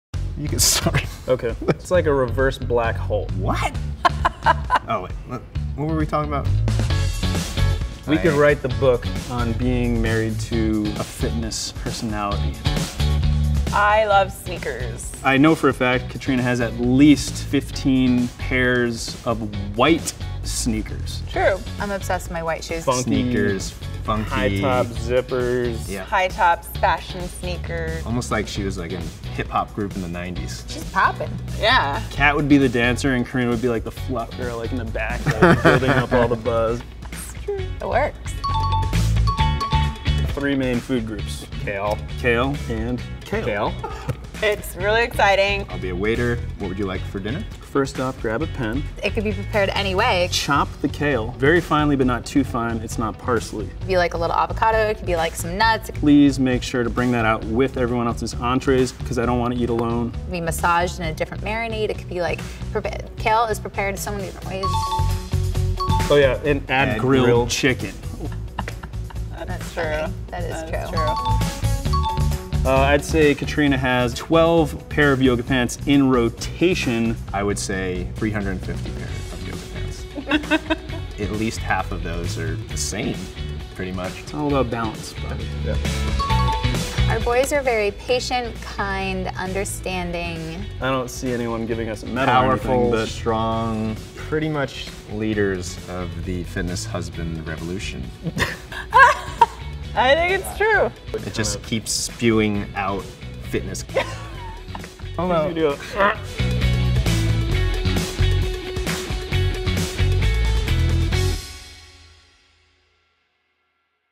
QALO | Tone It Up - Signs Your Partner Loves Health & Fitness
What does it take to be a TIUhusband? Bobby & Brian have it down! Watch as the Tone It Up Girls and their hubbys take us through some clear signs of being married to a health and fitness personality! Pro Tip: Be prepared to eat A LOT of kale.